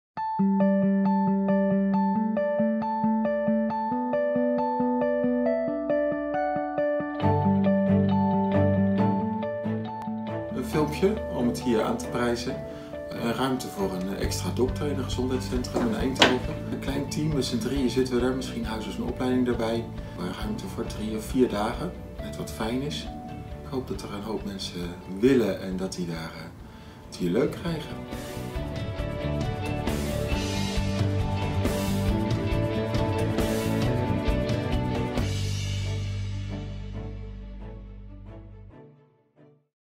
Onze deur staat open voor een huisarts!
Bij SGE De Akkers in Eindhoven zijn ze wegens het vertrek van één van de huisartsen op zoek naar aanvulling op het team: “Wij komen graag in contact met een bevlogen en betrokken huisarts”, vertelt clustermanager Petra Huijbregts. "En wat voor onze patiënten geldt, geldt ook voor jou: de deur staat open voor een warm welkom."